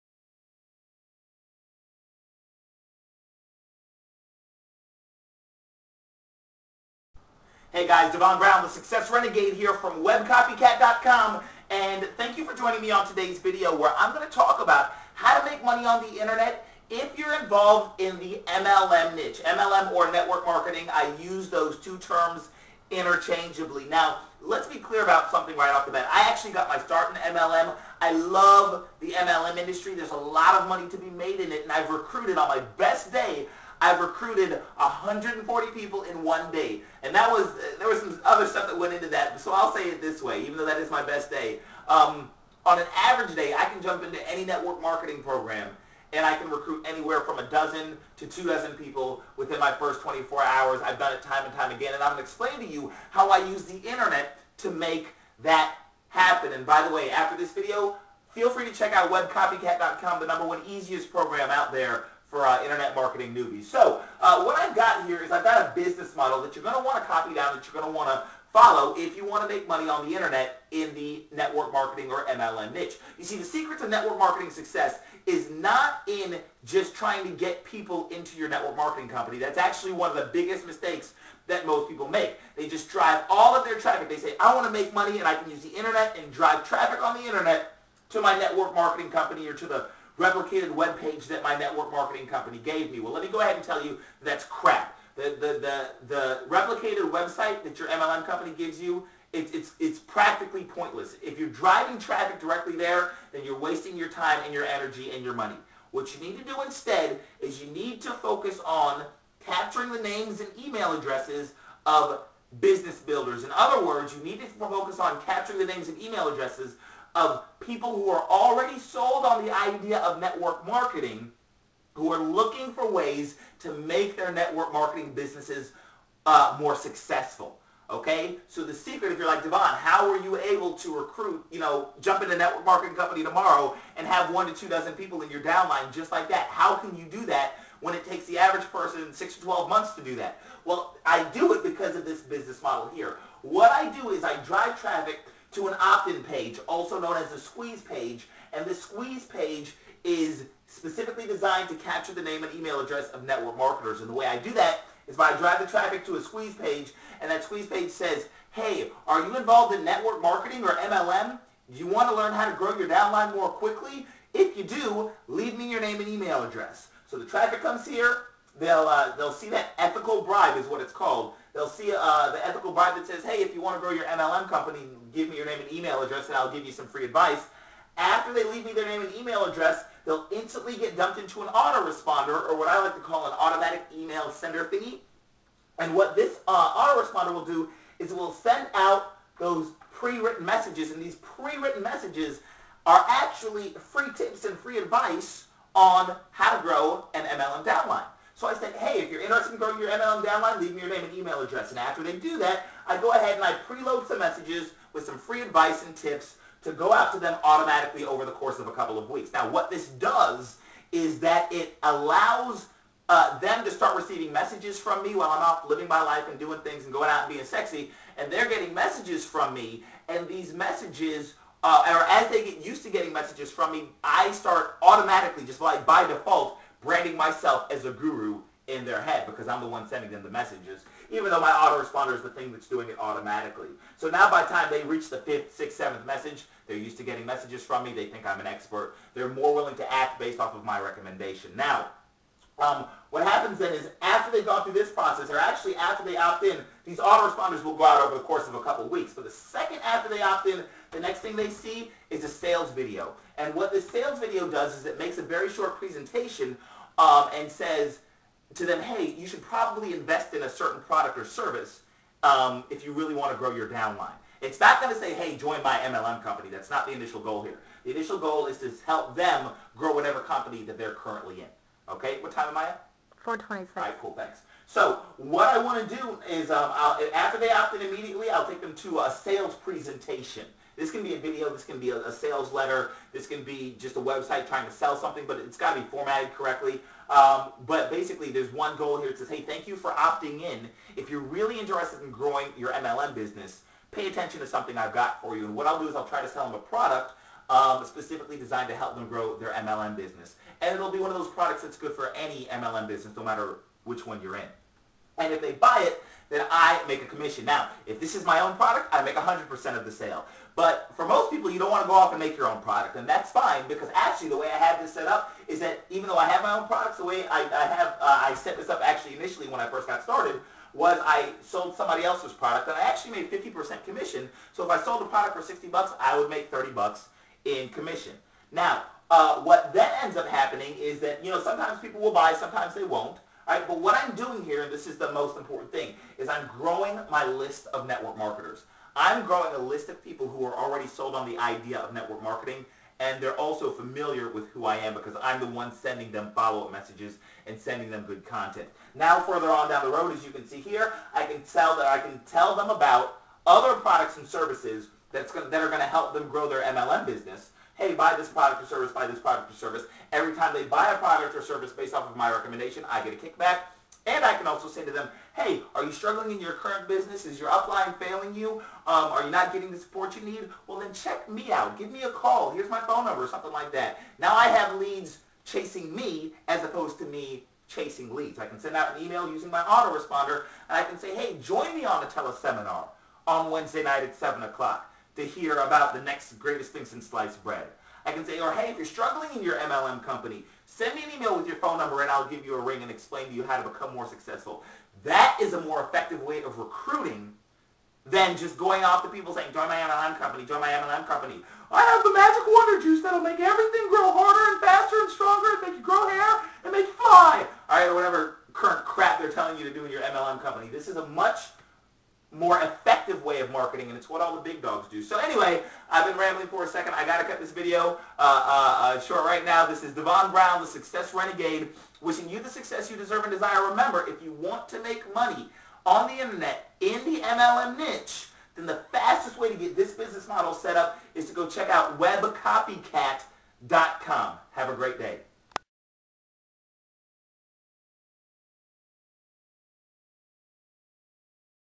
How to Create A Successful Internet Home Business: : MLM/Network Marketing Niche
How to Create A Successful Internet Home Business: MLM/Network Marketing Niche. Network marketing is a very popular niche and there is a lot of money to be made on the internet in helping people reach their MLM downline goals.